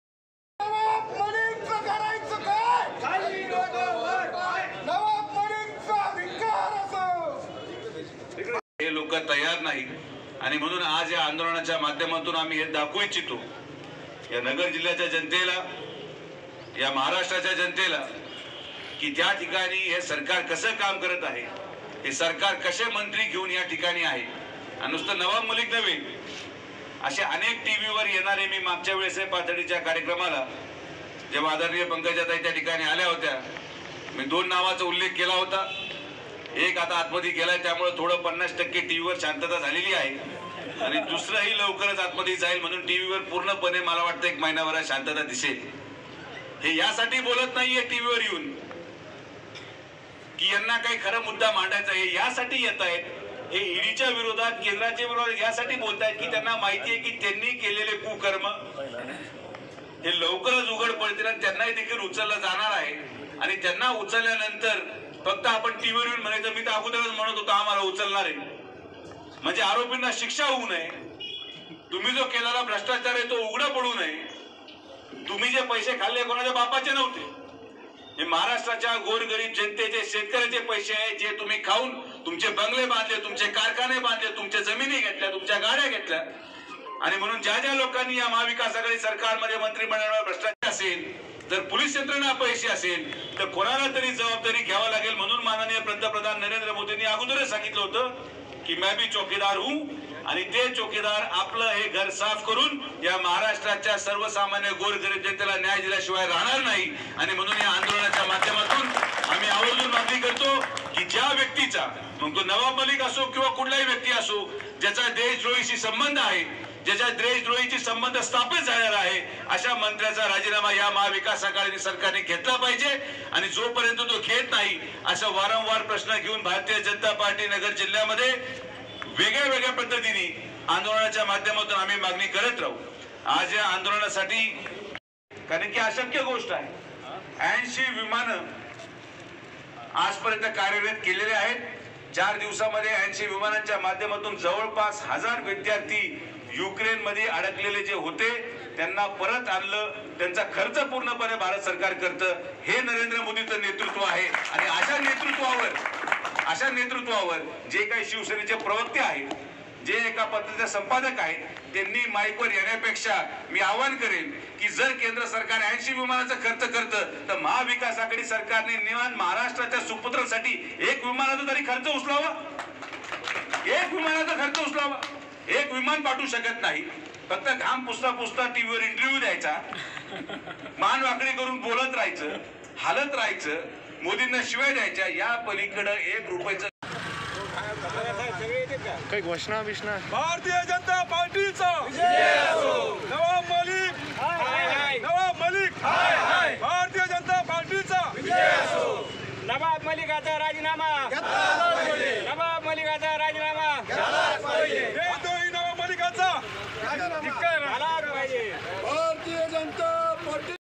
शेवगाव तहसील कार्यालयासमोर भाजपाचे धरणे आंदोलन BJP's protest in front of Shevgaon tehsil office
शेवगाव । राज्याचे अल्पसंख्याक मंत्री नवाब मलिक यांच्या राजीनाम्याच्या मागणीसाठी शेवगाव-पाथर्डी भाजपाच्यावतीने खा. सुजय विखे यांच्या नेतृत्वाखाली शेवगाव तहसील कार्यालयासमोर धरणे आंदोलन केले. यावेळी डॉ. सुजय विखे यांनी आघाडी सरकारवर टीका करत नवाब मलिकांच्या राजीनाम्याची मागणी केली.
Shevgaon-Pathardi on behalf of the BJP to demand the resignation of Nawab Malik, the state's minority minister. Under the leadership of Sujay Vikhe, a protest was held in front of Shevgaon tehsil office. At this time, Dr. Sujay Vikhe criticized the alliance government and demanded the resignation of Nawab Malik.